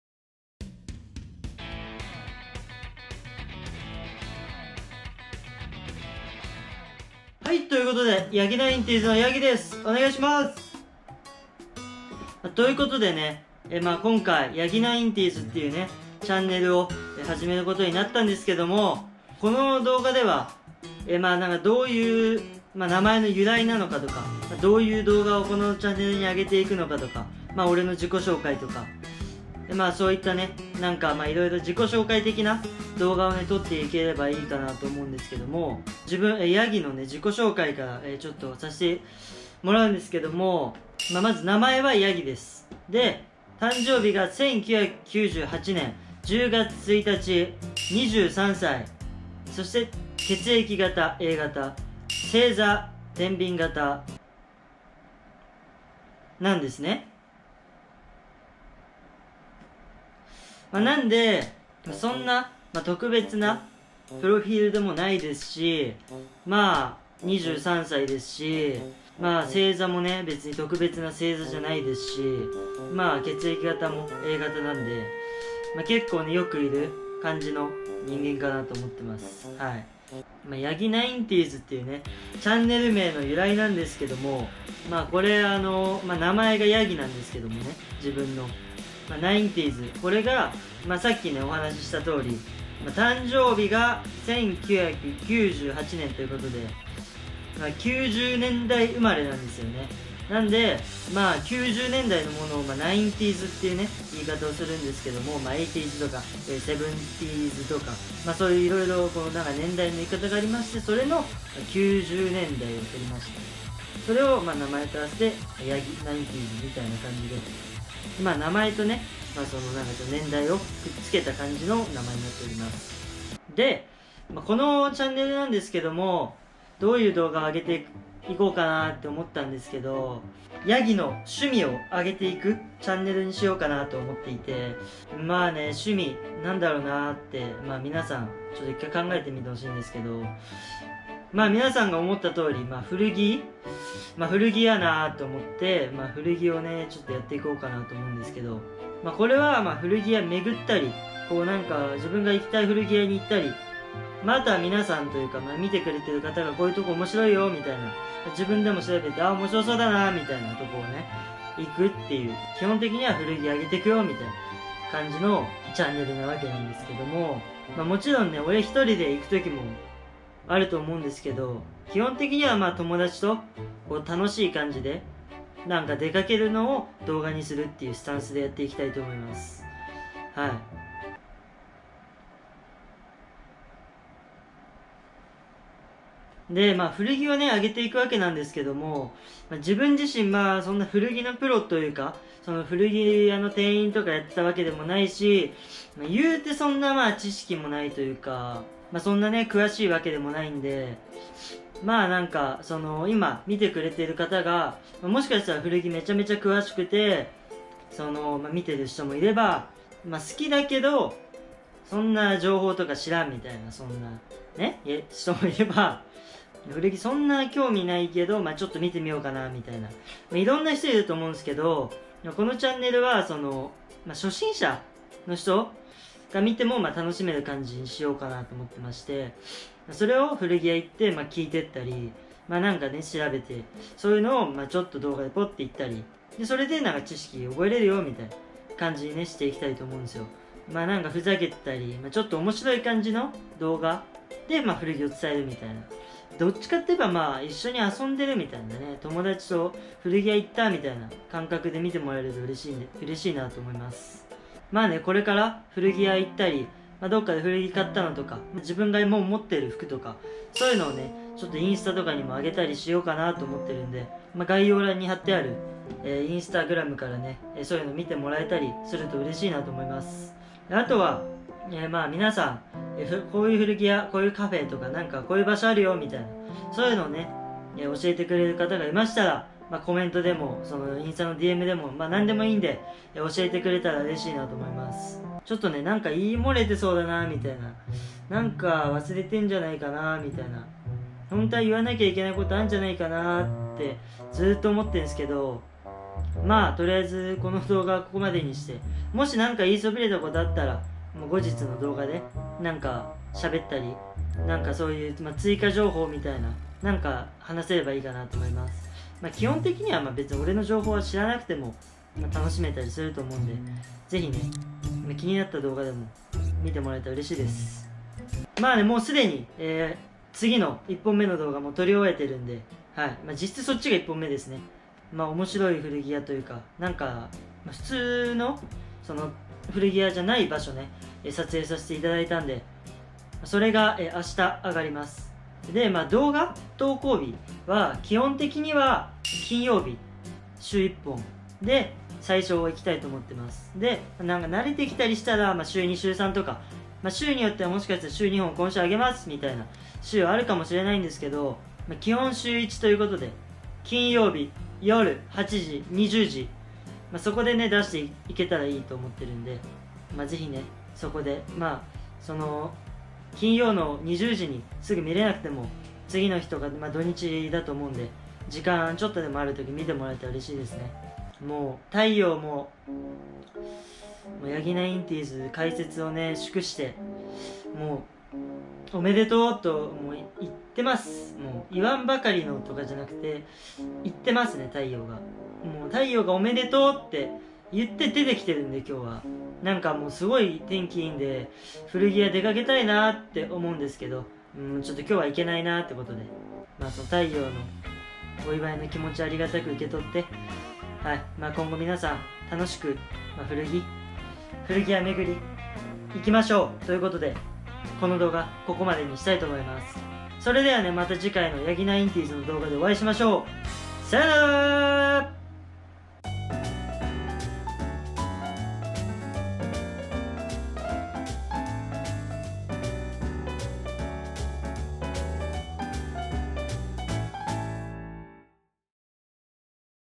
【初投稿】古着好きがYouTube始めます！
古着好きがYouTubeを始めることにしました。
ですが、正直そんな知識もなければ詳しくもありません。
なので楽しく緩く古着屋を巡ったりカフェに行ったりするのを見てもらえたら嬉しいです。
自分が知ってる事や古着屋さんで聞いた知識など知っていたらより古着が楽しめる情報を少しずつお伝えしていきます。

とにかく楽しみながらどこかに出かけて行きます！